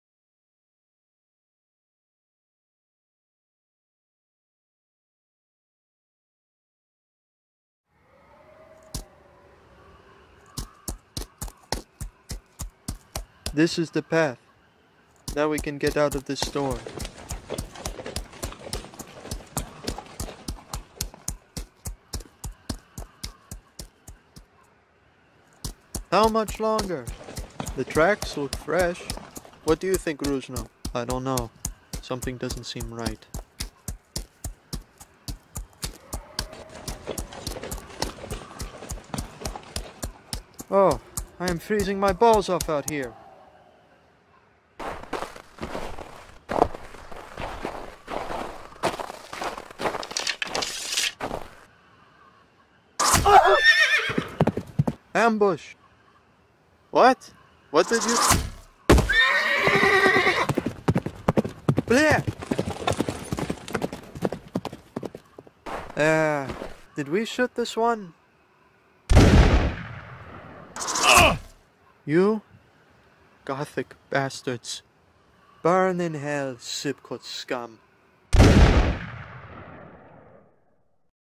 Castle 9 Season 1 Episode 1 Teaser Trailer (Lego Animation) (Not for Kids)
Castle 9 is a story that I've been writing with some friends that focuses on the rise and fall of nations across a world full of war. Nishtruvisk is the first continent that will be explored, which is the Northern ice cap of the planet, and although it is habitable enough to support life, it is a struggle to survive.

The story will explore the rise of The Black Cross, a Gothic nation as it seeks independence from the Sibkut Empire, which dominates the entire continent, and hasn't lost a conflict in over 200 years.

I cannot give an exact time when I will be done with any of this series, but somewhere around 2022 is the best guess I can give.